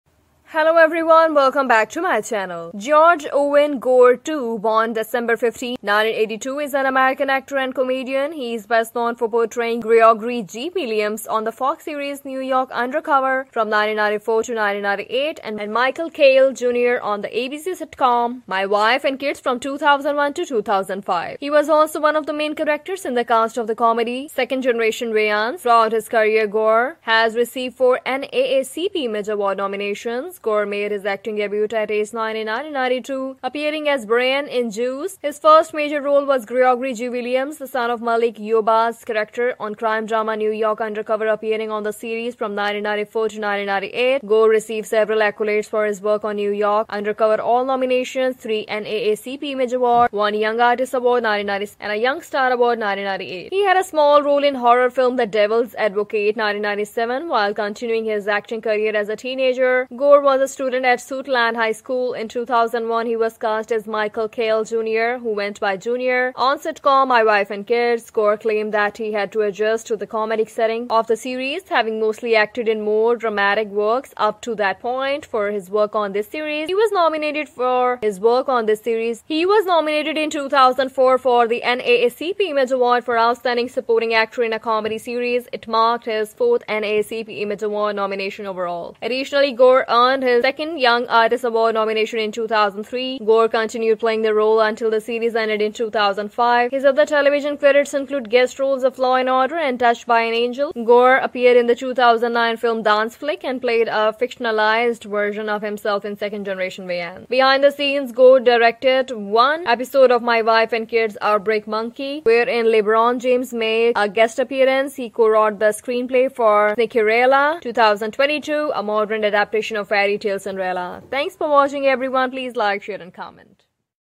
George O. Gore II's Wife, Net Worth, Mansion & More (BIOGRAPHY)
George Owen Gore II (born December 15, 1982) is an American actor and comedian. He is best known for portraying Gregory "G" Williams on the FOX series New York Undercover from 1994 to 1998 and Michael Kyle, Jr. on the ABC sitcom My Wife and Kids from 2001 to 2005.